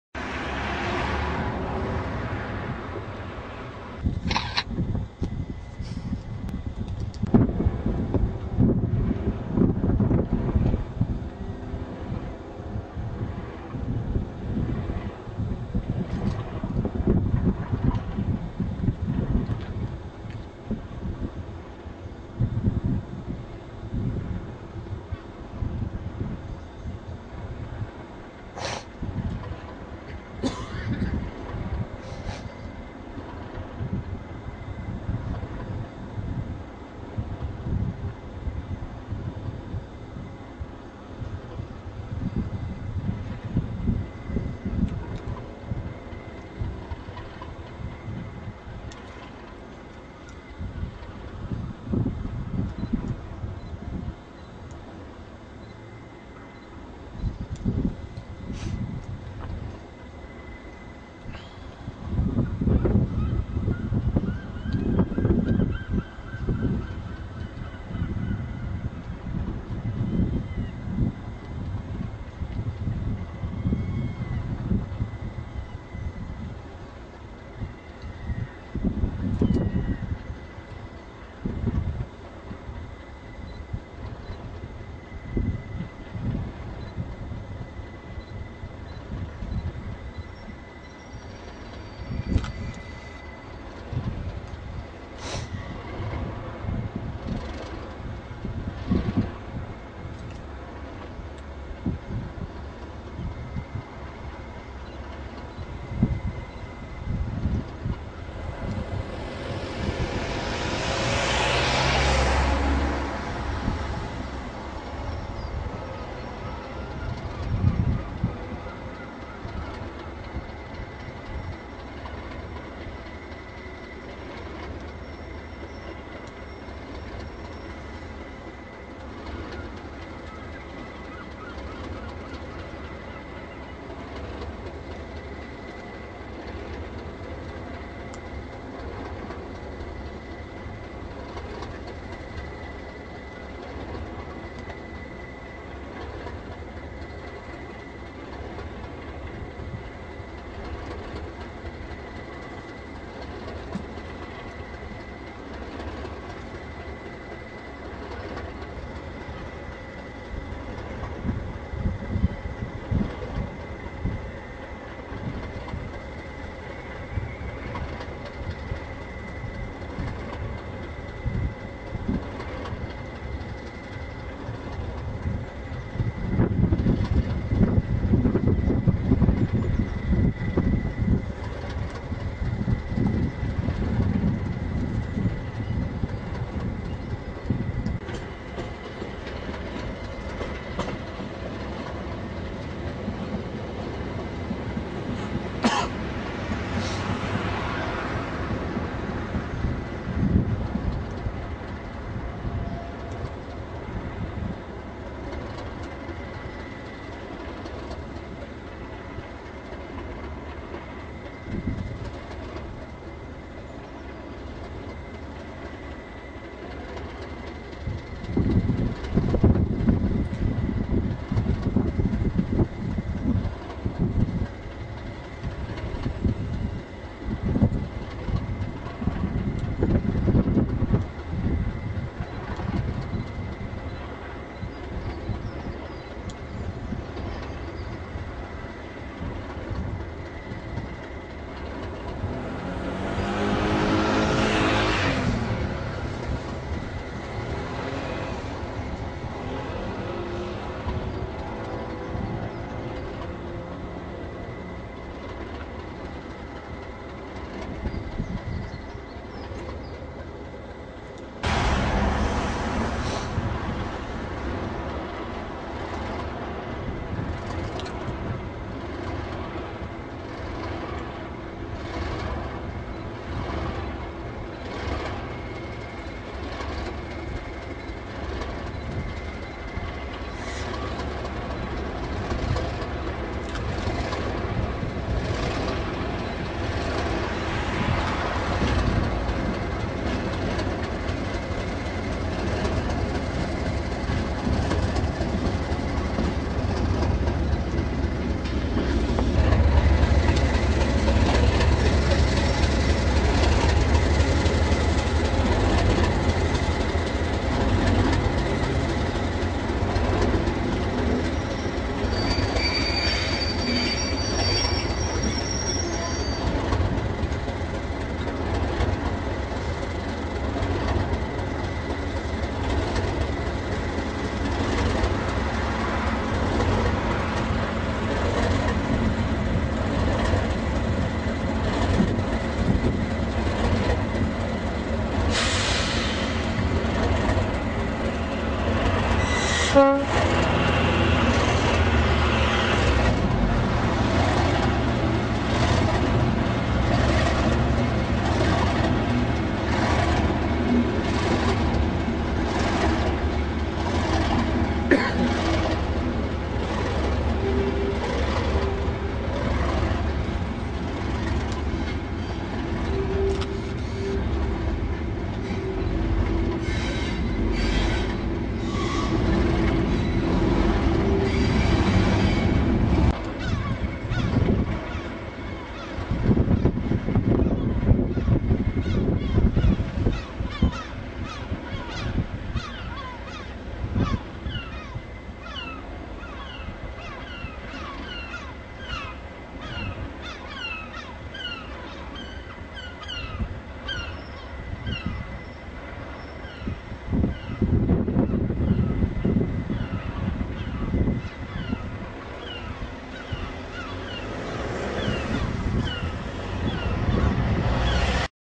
37611 & 37602 at Brighton Lovers Walk Depot
DRS 37611 & 37602 at Brighton Lovers Walk Depot in 2008.